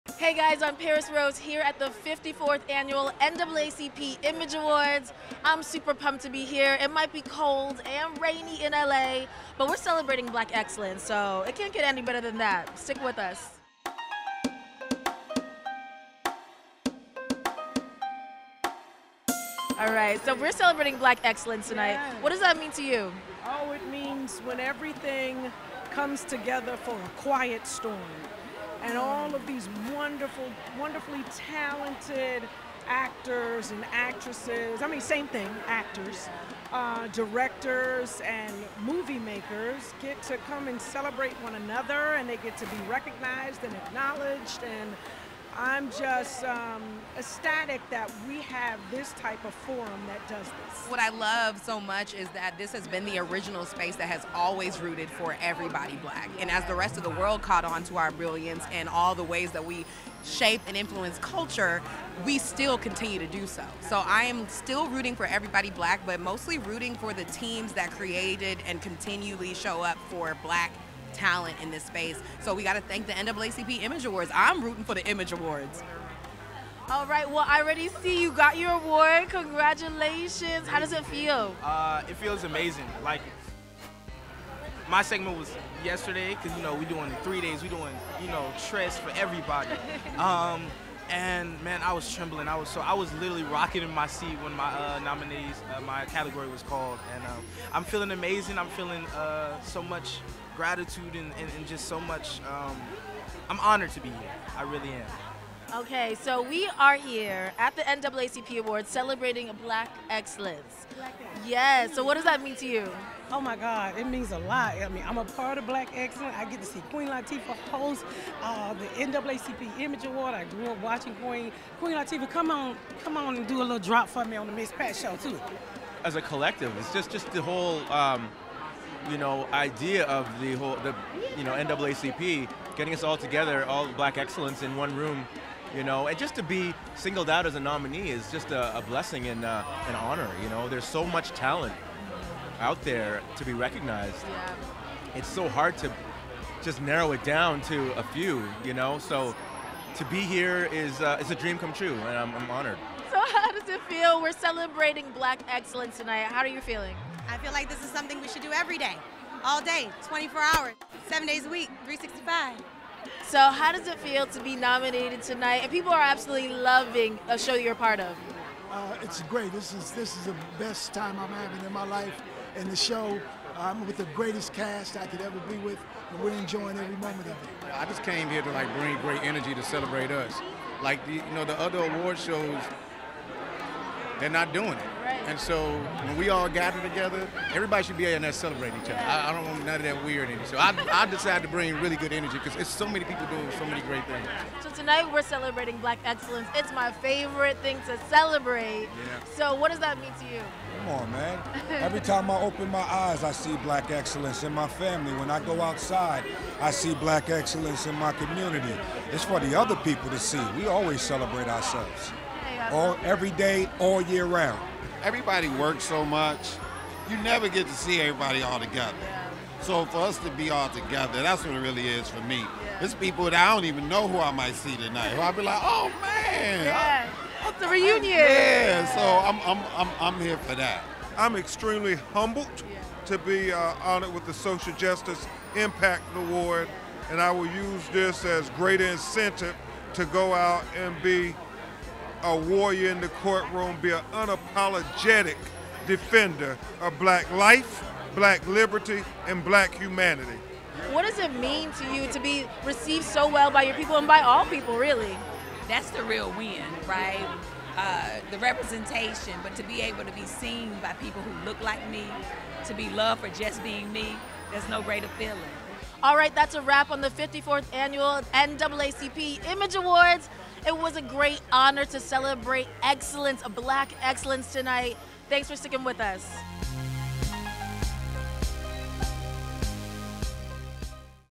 Method Man, Ms. Pat & Adrian Holmes Hit The Image Awards Red Carpet! | NAACP Image Awards '23
Tap in with BET Correspondent Parris Rose as she interviews a whole host of celebs including: Method Man, Ms. Pat, Adrian Holmes, Jalyn Hall, and many other stars on the red carpet at the 2023 NAACP Image Awards.